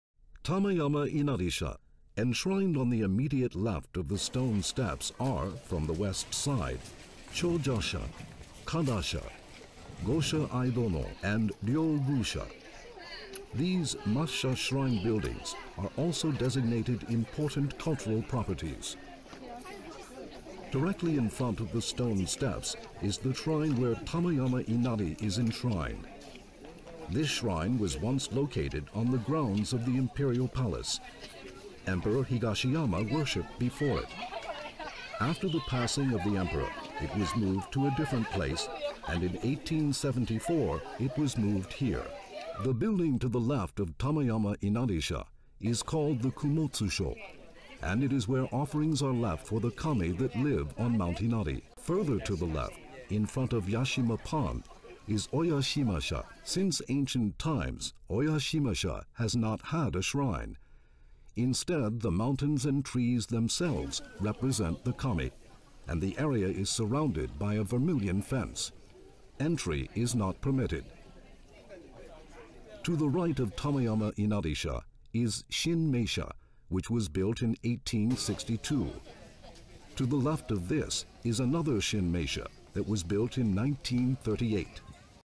Tamayama Inarisha, Kamimassha Inari AUDIO GUIDE(KYOTO)
Enshrined on the immediate left of the stone steps are, from the west side, Chojasha, Kadasha, Gosha Aidono, and Ryōgūsha. These Massha shrine buildings are also designated important cultural properties.
Directly in front of the stone steps is the shrine where Tamayama Inari is enshrined. This shrine was once located on the grounds of the Imperial Palace. Emperor Higashiyama worshipped before it. After the passing of the emperor, it was moved to a different place, and in 1874 it was moved here.

The building to the left of Tamayama Inarisha is called the Kumotsusho, and it is where offerings are left for the kami that live on Mt. Inari

Further to the left, in front of Yashima Pond is Oyashimasha. Since ancient times, Oyashimasha has not had a shrine. Instead, the mountains and trees themselves represent the kami, and the area is surrounded by a vermilion fence. Entry is not permitted.

To the right of Tamayama Inarisha is Shinmesha, which was built in 1862. To the left of this is another Shinmesha that was built in 1938.